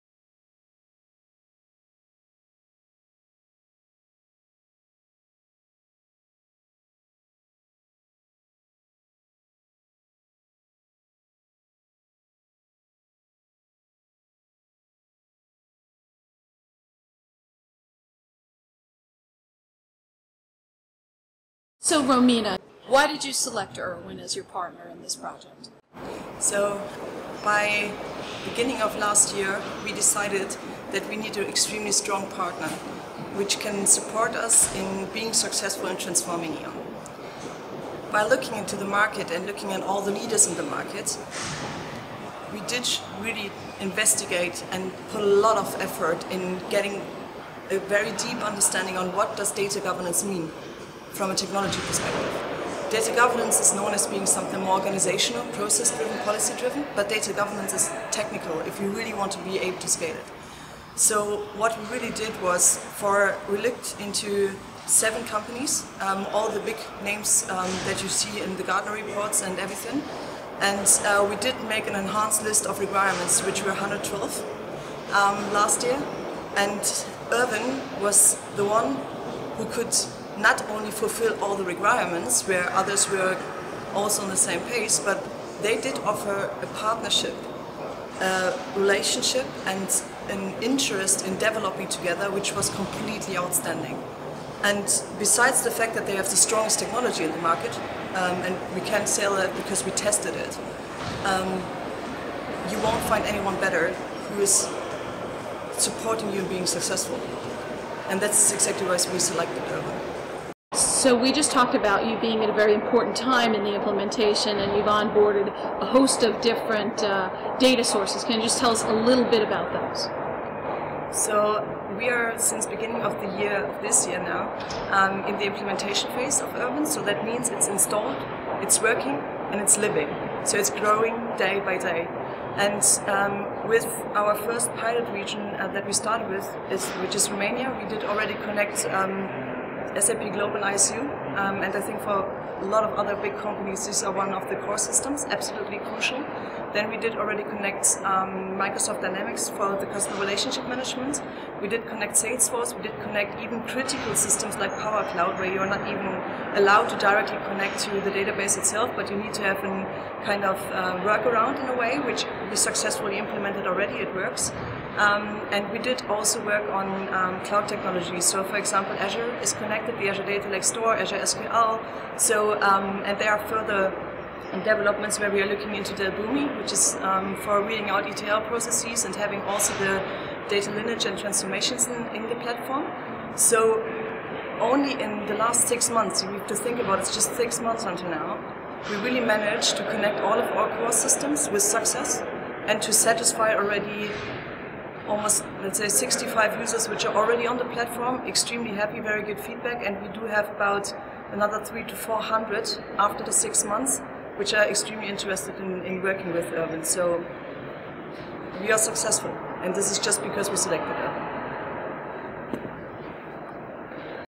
E.ON Energy: Business Transformation and Data Readiness
E.ON’s Data Manager Romina Medici explains the organization’s progress after six months of using the erwin Data Intelligence Suite. The global energy provided selected erwin DI as it data governance technology platform and began its implementation with a pilot in Romania, where all core systems have been integrated successfully.